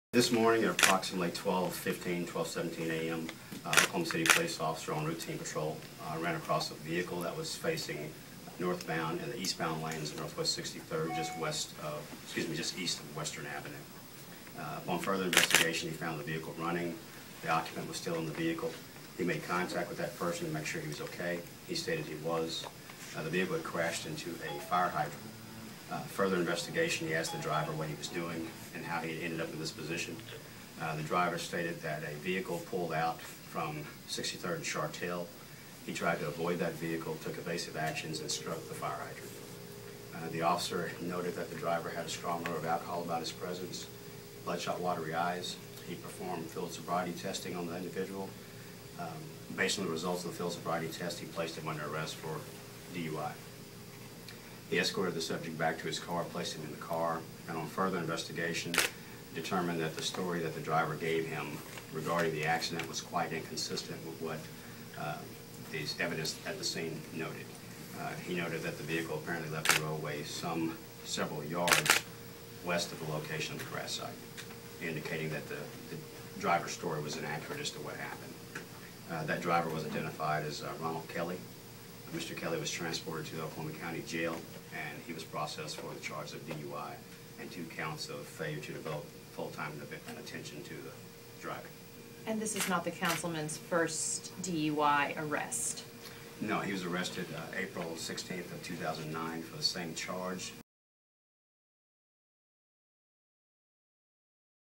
Councilman Skip Kelly arrested (2012-01-20)
Oklahoma City Councilman Skip Kelly was arrested on drunken driving complaint. He was released from the Oklahoma County jail Friday morning on $750 bail